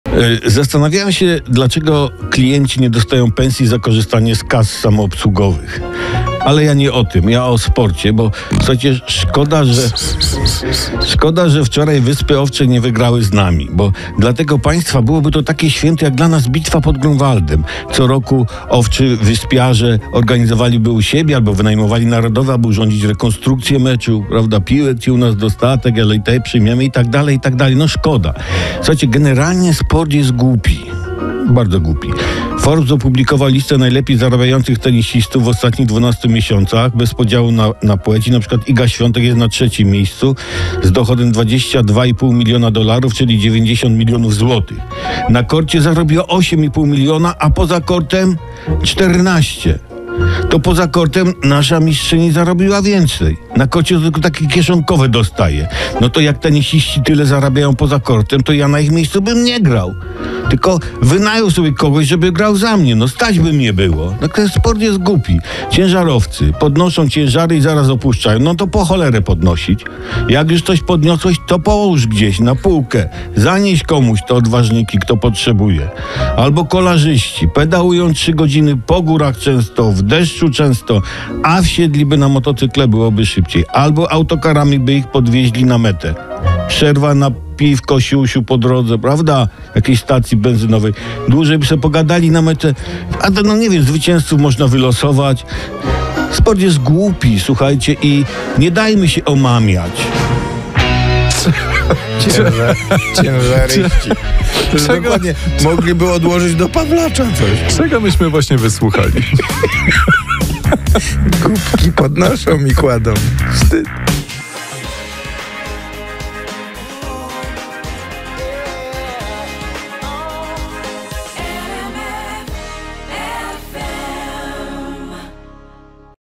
G(ł)upi sport - (08.09.2023) Felieton Tomasza Olbratowskiego
Zastanawiałem się dlaczego klienci nie dostają pensji za korzystanie z kas samoobsługowych. Ale ja nie o tym. O sporcie. Szkoda, że wczoraj Wyspy Owcze nie wygrały z nami. Dla tego państwa byłoby to takie święto jak dla nas Bitwa pod Grunwaldem. Co roku Owczywyspiarze organizowaliby u siebie, albo wynajmowali Narodowy, by urządzać rekonstrukcję meczu. Piłek ci u nas dostatek, ale i te przyjmiemy – i tak dalej. No szkoda. Generalnie sport jest głupi...